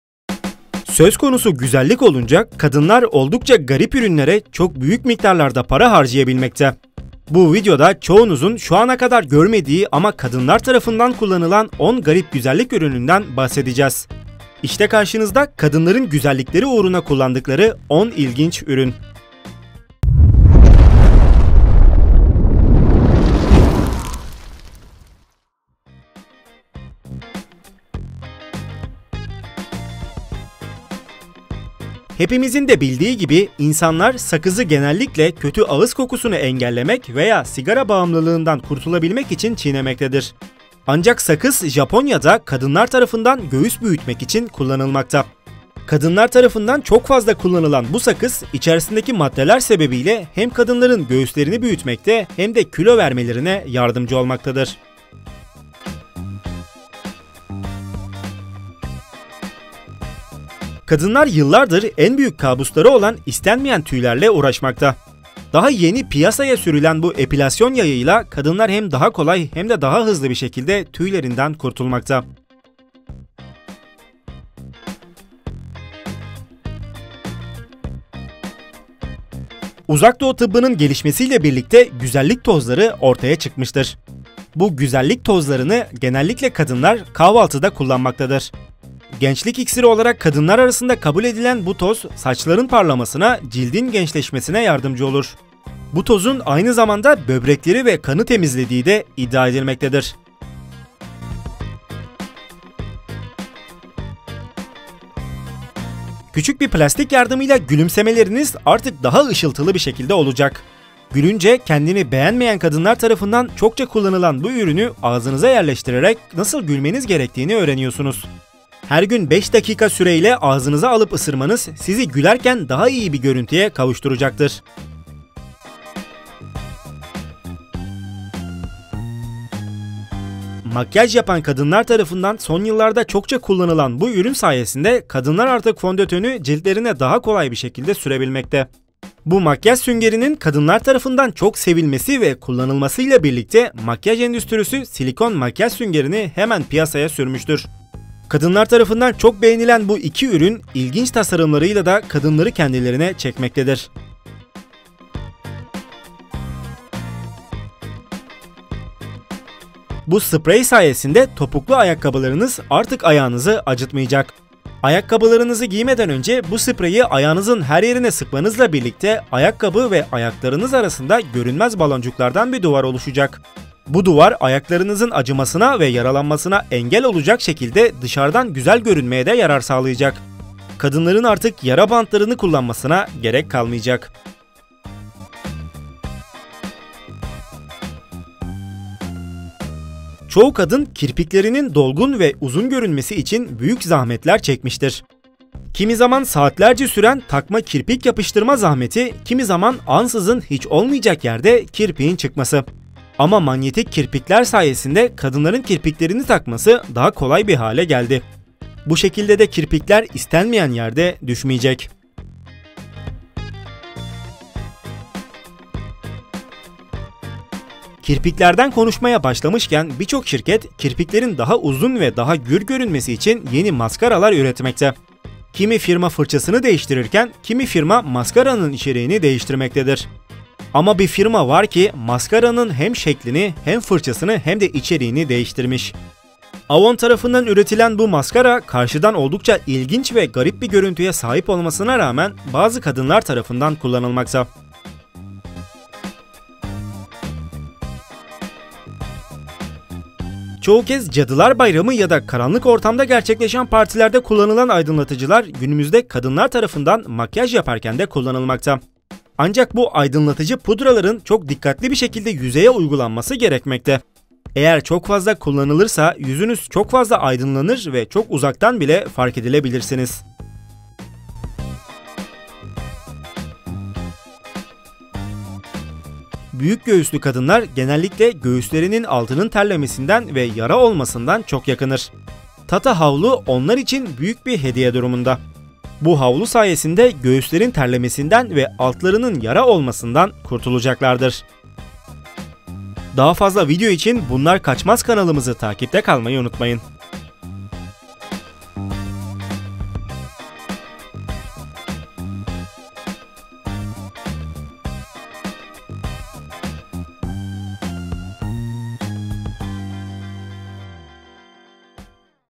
Kadınların Paralarını Harcadığı GERÇEKTEN Garip 10 Ürün
Kadınların kullandığı birbirinden ilginç ürünler.
Bu videoda sizler için videoda kadınların kullandığı birbirinden garip ve ilginç ürünleri inceledik.

Bunlar KAÇMAZ!